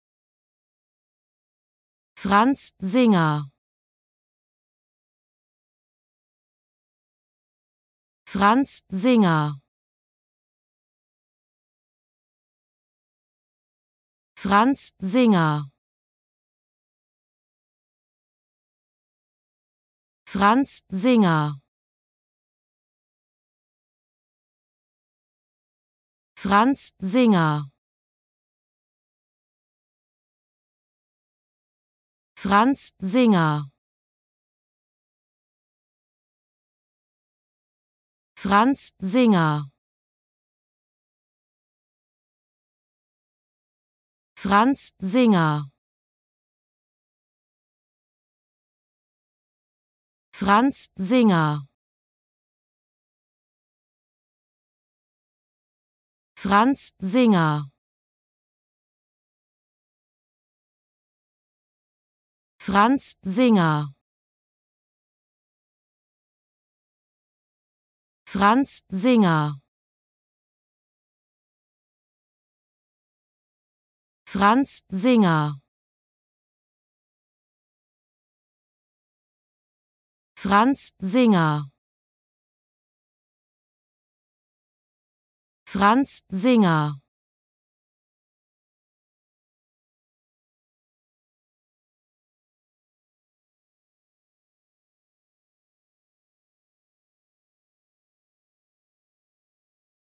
How to Pronounce ''Franz Singer'' Correctly in German
Learn how to say ''Franz Singer'' in German with this short pronunciation tutorial.